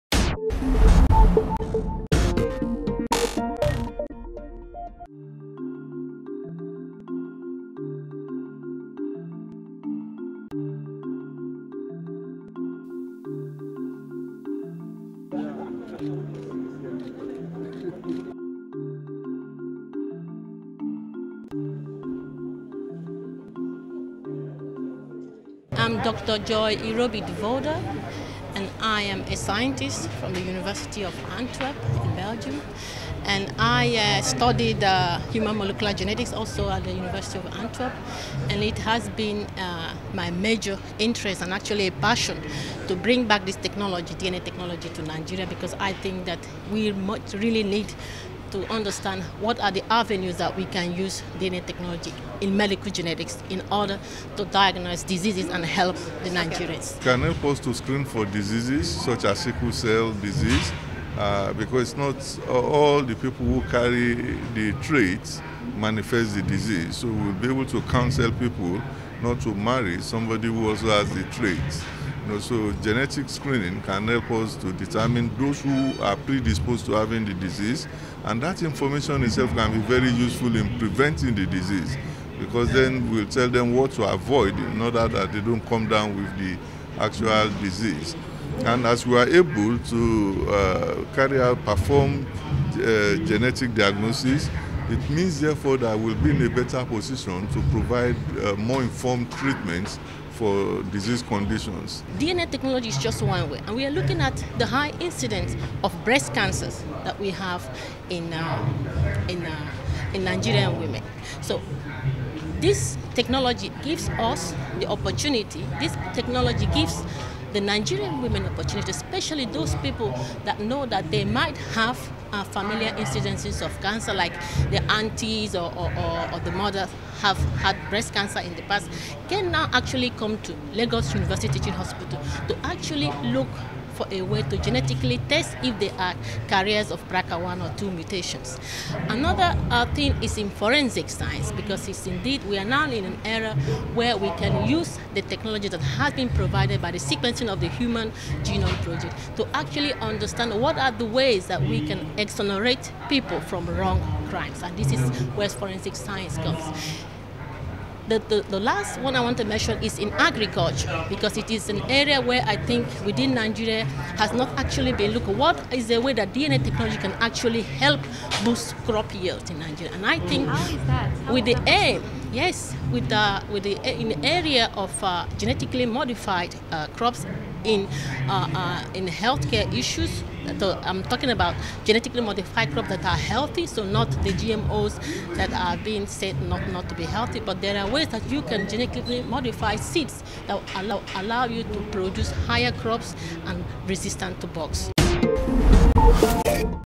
Unlocking DNA technology in Nigeria
In this video, discover how a new molecular biology laboratory could boost the nation's genetic research.